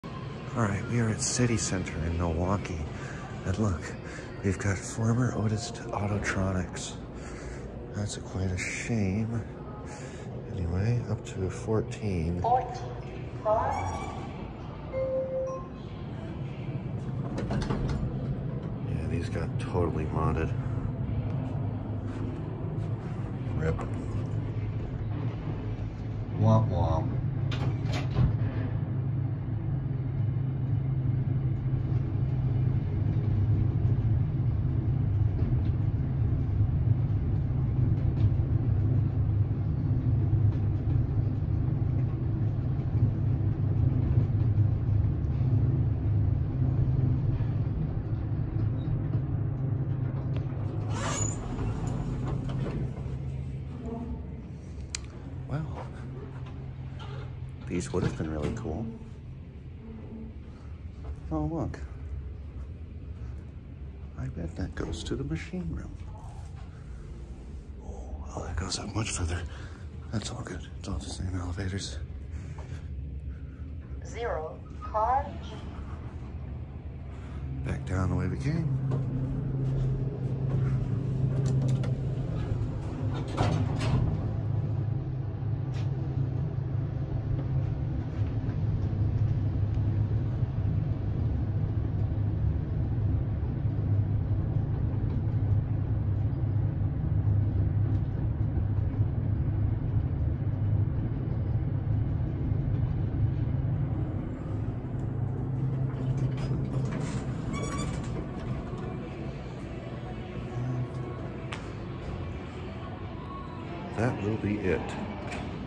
Otis/Schindler Elevators @ CityCenter - Milwaukee, WI.
TECH SPECS:
Brand: Otis
Modernized By: Schindler
Model: Port
Drive: AC Gearless Traction
Year Installed: 1950s
Floors Served: 14
Capacity: 2250 lbs
Speed: 700 FPM
Doors: Center Opening

FIXTURES:
Buttons: Adams Survivor
Indicator: Epco Segmented DPI